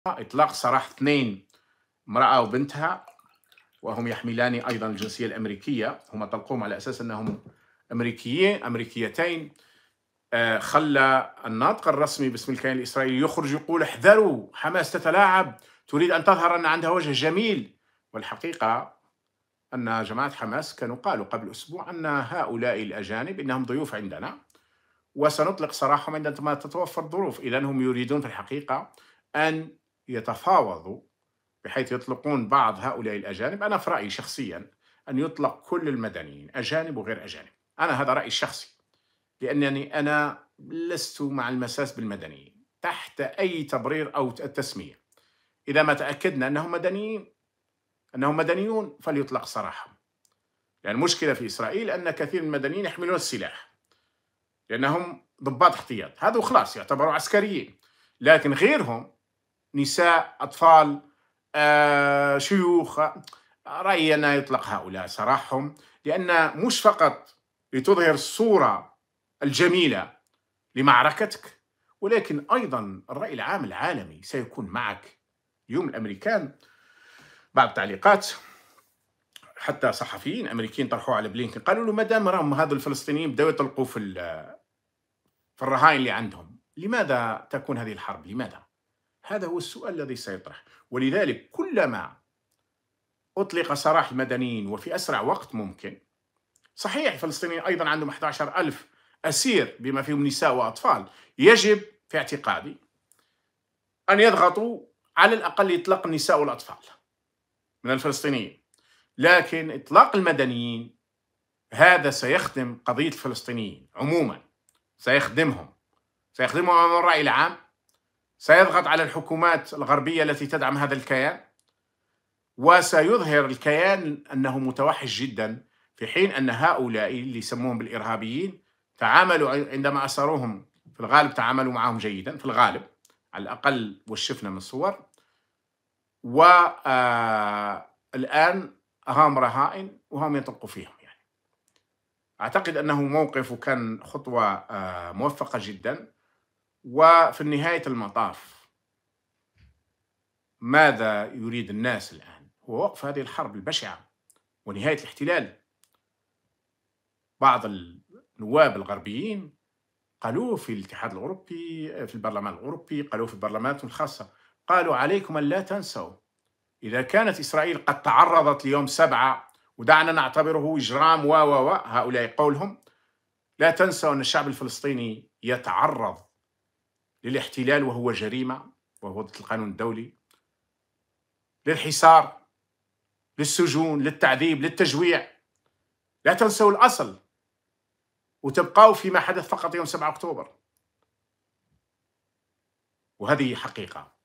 حماس تقوم بـ إطلاق سراح رهينتين أمريكيتين...
محمد العربي زيتوت 20 أكتوبر 2023